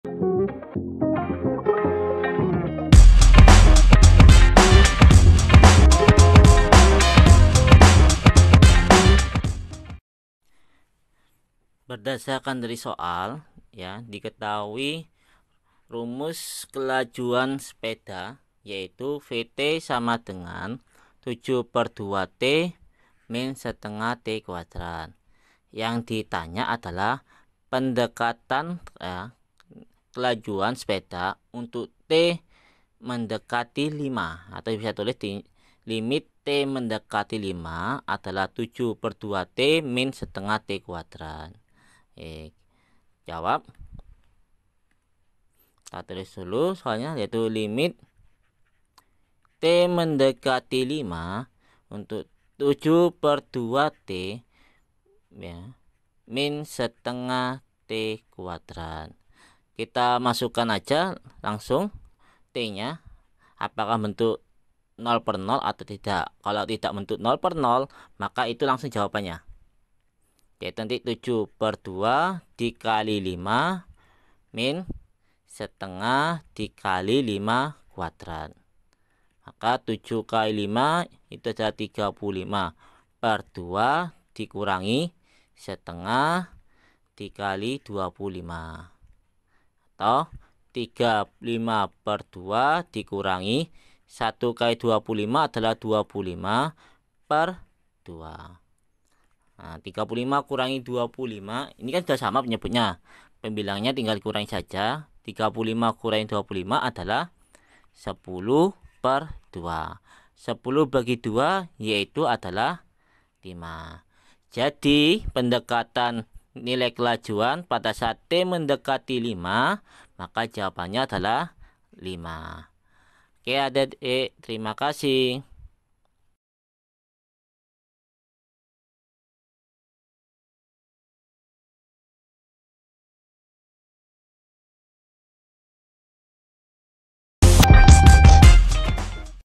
Sebuah sepeda melaju dengan kelajuan setia... | PR Matematika U | Kelas 11 | 1
✅ Dalam video ini kita akan membahas: Sebuah sepeda melaju dengan kelajuan setiap saat dirumuskan dengan  (t)=\frac{7}{2}t-\frac{1}{2}t^2 , v dalam m/detik dan t dalam detik.
Tentukan nilai pendekatan kelajuan sepeda tersebut untuk t mendekati 5 detik.
Tema:
Kelas: 11
Mata Pelajaran: Matematika
Bab: 1
Buku: PR Matematika Untuk SMA/MA Mata Pelajawan Wajib Kelas XI Semester 2

👉 Buku yang telah tersedia:
{PLAYLIST_LINK}
Di mana kamu dapat menemukan kami?